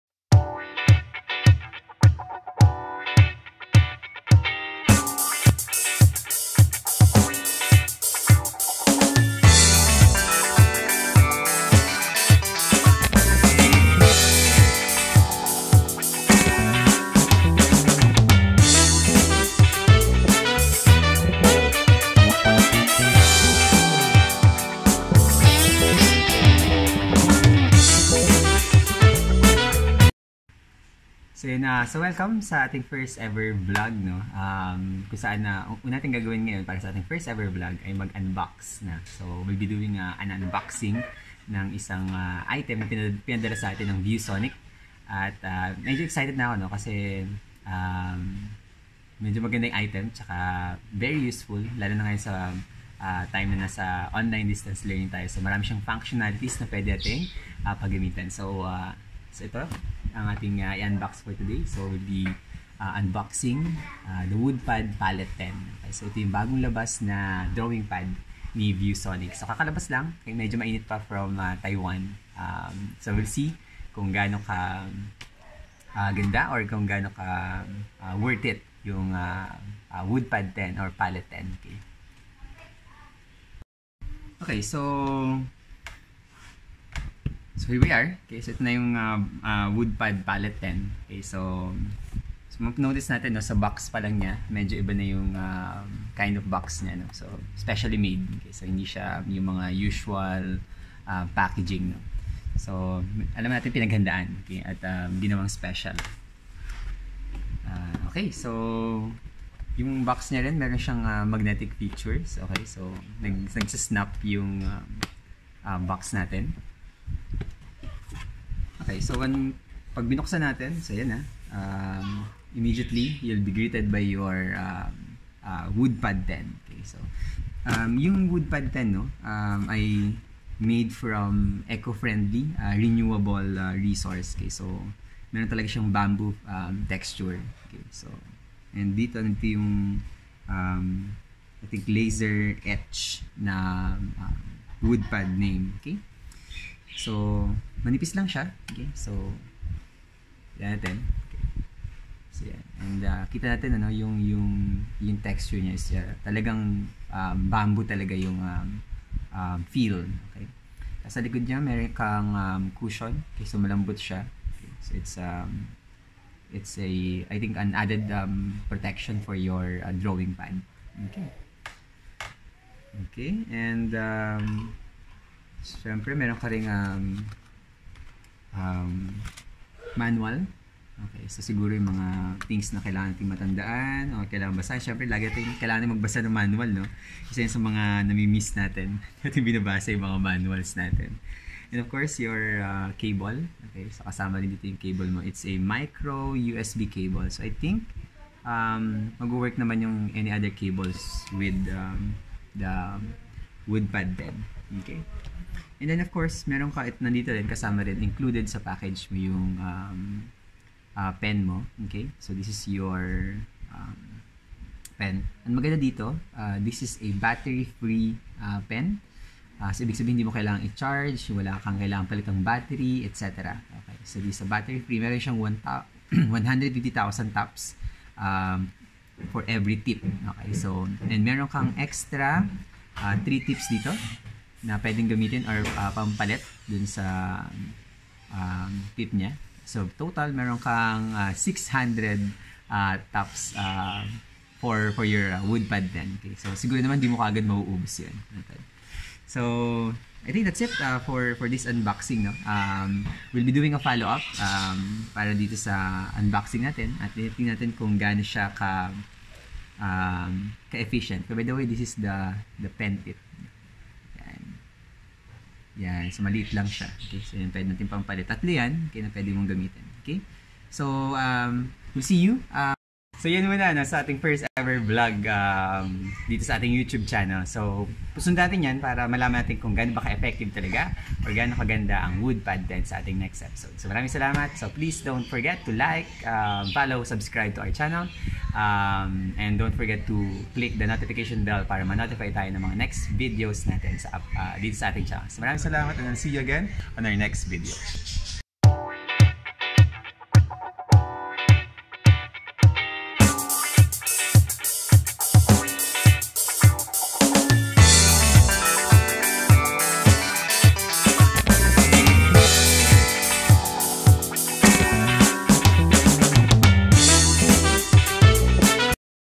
VLOG #1: My First EVER VLOG! Unboxing of Woodpad 10
Nagpadala si Viewsonic Ph ng woodpad! Ito na na-unbox na finally! 😊

Salamat Viewsonic ph for this woodpad 10!

✔️ 5080 LPI (lines per inch)
✔️ 250 pps
✔️ Lightweight and portable
✔️ Ecofriendly and renewable materials, bamboo finish

Available in Woodpad 7 and 10! 😊